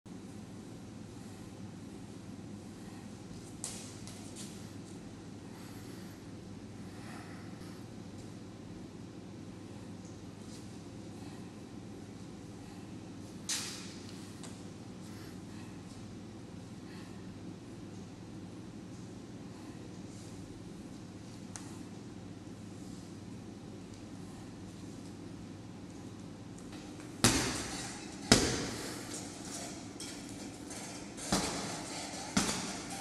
"Hammer" blow 6. Боковой удар.
Некоторые варианты нанесения удара подобного ударам молотком.